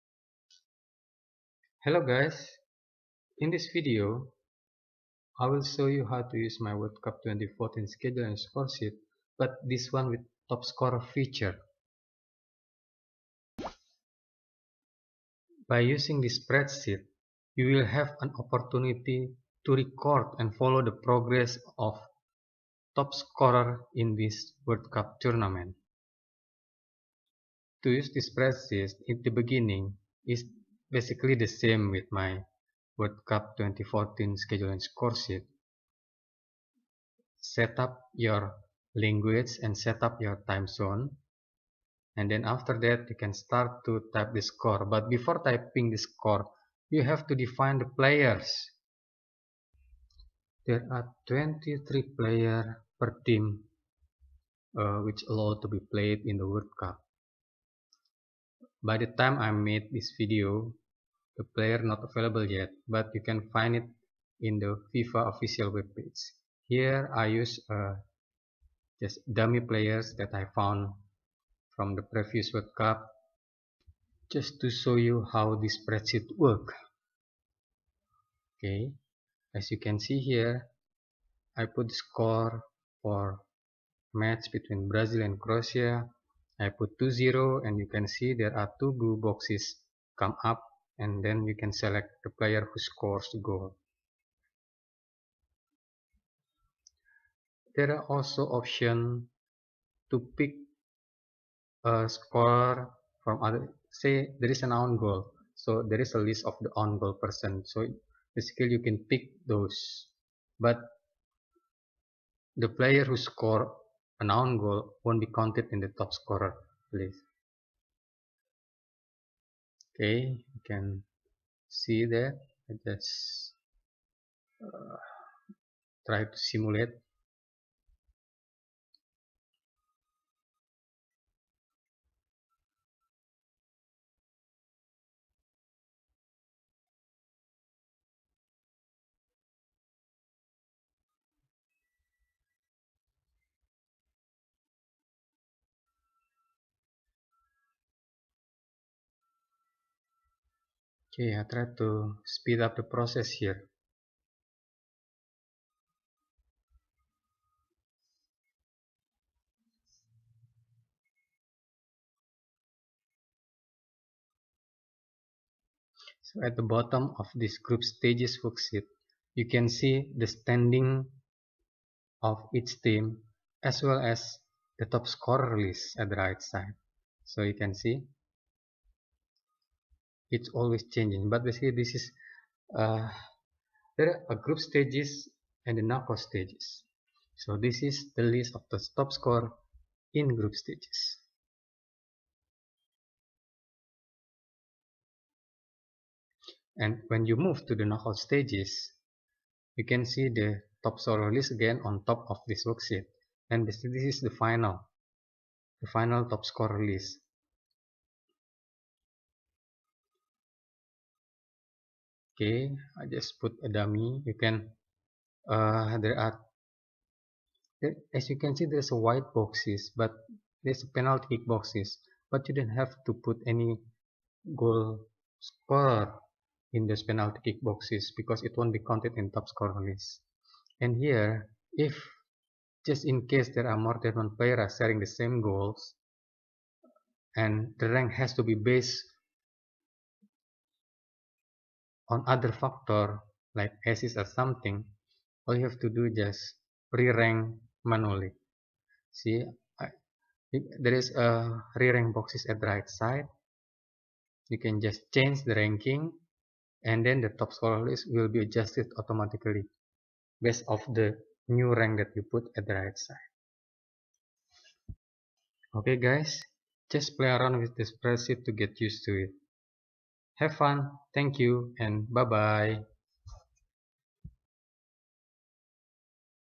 World Cup 2014 Scoresheet with Top Scorer Feature
- This is a simple video to guide you on using a World Cup 2014 Scoresheet with Top Scorer Feature created using MS Excel. This spreadsheet will allow you to track the progress of any team in World Cup 2014, held in Brazil, as well as its goal scorers. All you have to do just typing the score and selecting goal scorers after every matches is completed, and let excel formula do all rankings stuff. And you can put your own score and goal scorer before the matches are played as part of your own tournament simulation.